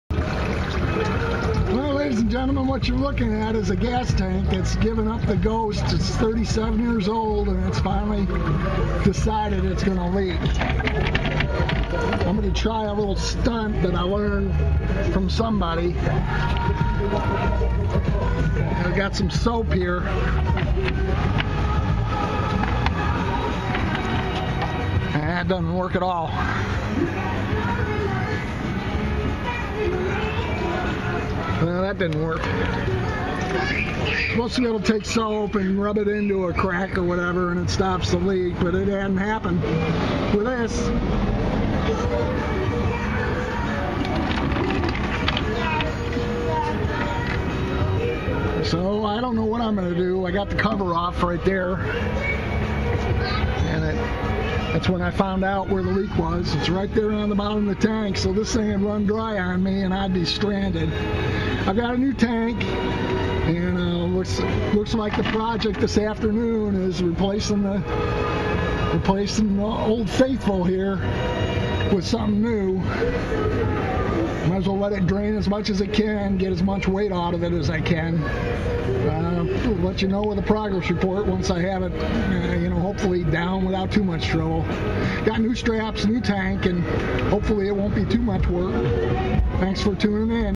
1981 C10 fuel tank geyser 02
I was wondering why gas mileage was worse than bad.  The tank had been seeping for awhile, and it finally let go.  37 years later, I was glad I was able to find a replacement unit.